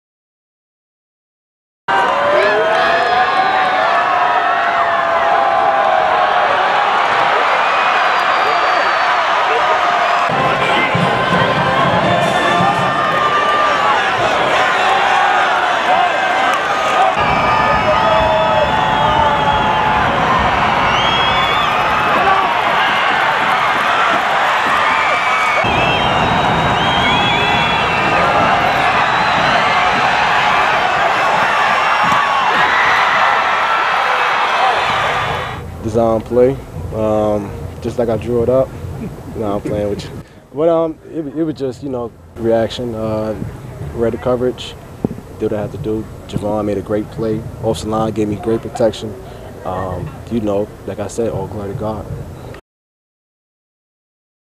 De'Andre Johnson jolts FAU offense to life in Shula Bowl
FAU QB De'Andre Johnson, of Last Chance U fame, came off the bench to rush for 50 yards and threw two TDs in an Owls in over FIU in the Shula Bowl.